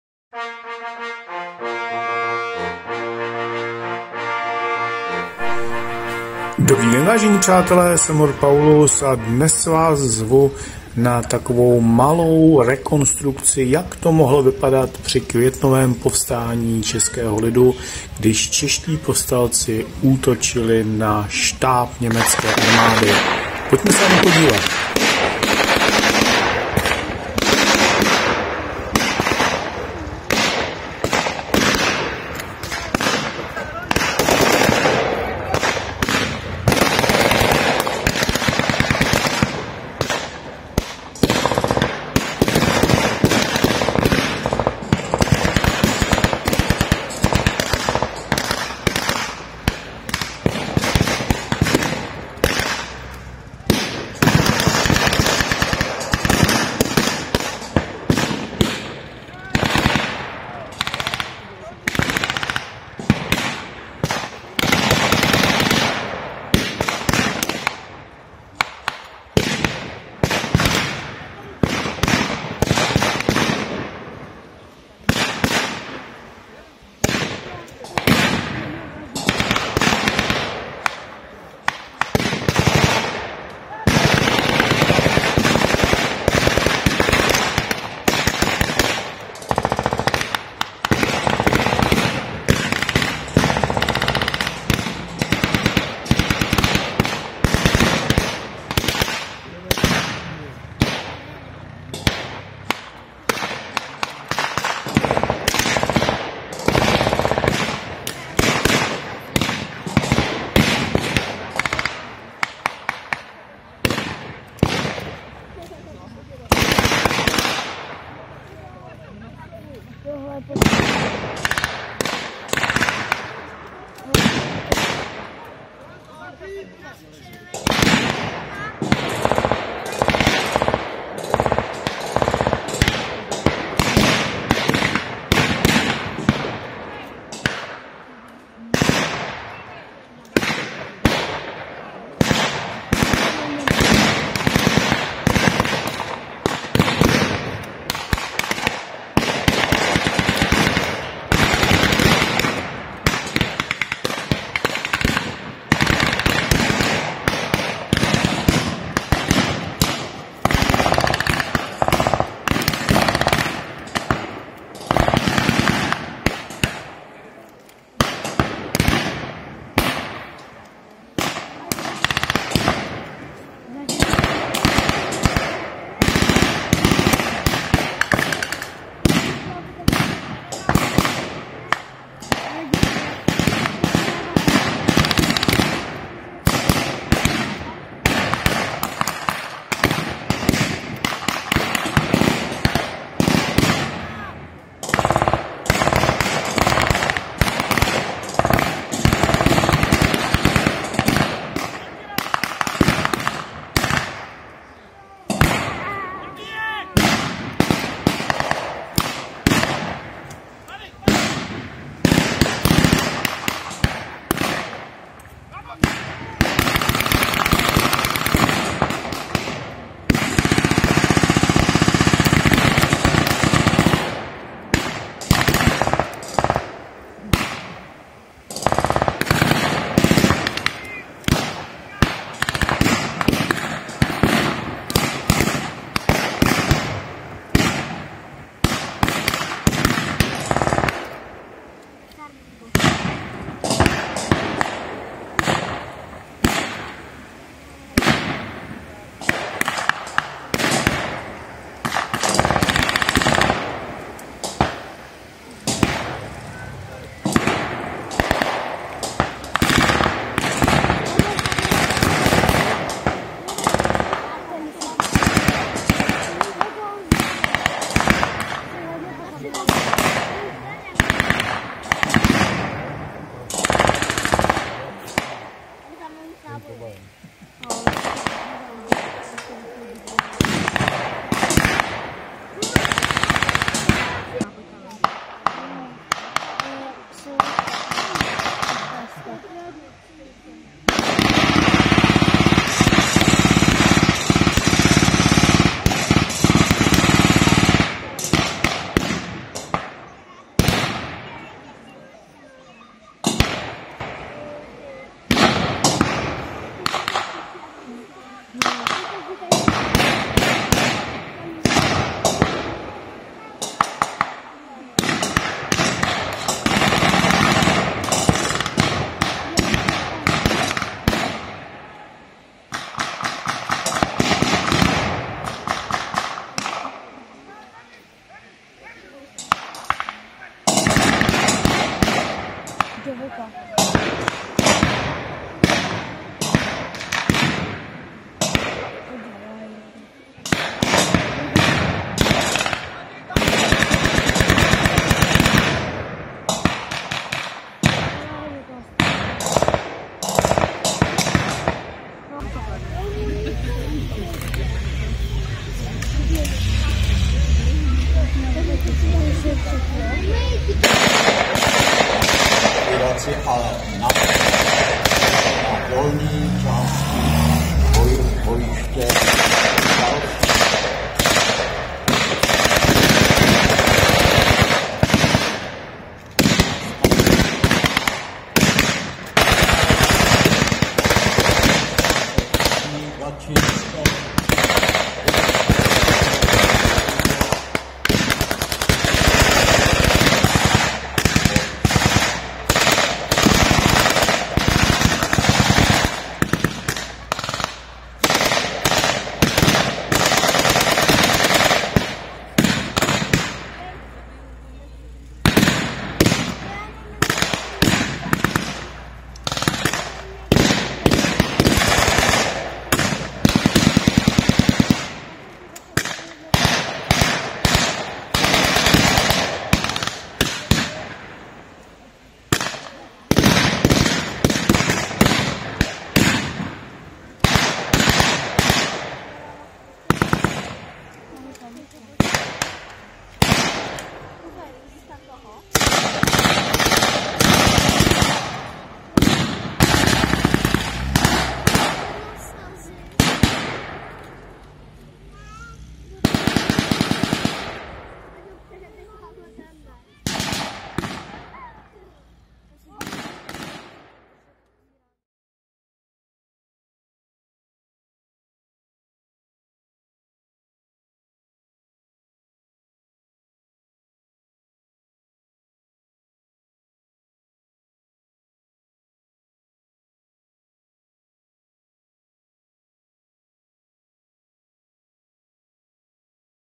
Květen 1945: Útok českých povstalců na německý štáb a příchod Rudé armády
Jak to mohlo vypadat při Květnovém povstání českého lidu v roce 1945? Podívejte se na ukázku inspirovanou skutečnými událostmi, které se odehrává v nejmenovaném českém městě.

Ukázka proběhla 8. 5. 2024 v rámci akce Mise Velichovky v blízkosti tzv. Schörnerovy vily. Dne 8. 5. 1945 zde byla polnímu maršálovi Ferdinandu Schörnerovi, veliteli německých armád skupiny "Střed" (Mitte) předána vojáky americké armády informace o kapitulaci německých ozbrojených sil. Pro více informací o této důležité a pro mnohé neznámé události hledejte termín MISE VELICHOVKY.